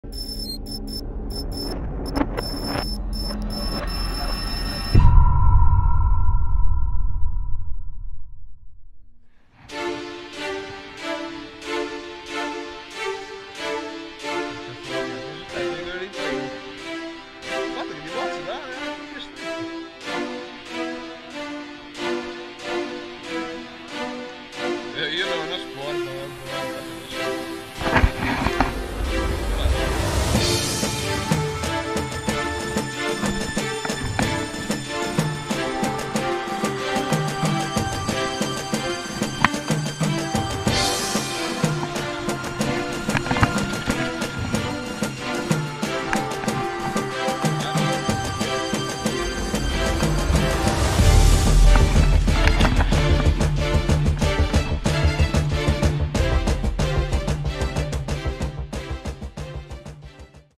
STAN LURE - ALICE tuna stick bait su alletterato
Stick bait handmade di casa STAN LURE.
Corpo in resina con armatura passante in acciaio inox da 1,6mm e finitura in resina epossidica resistente.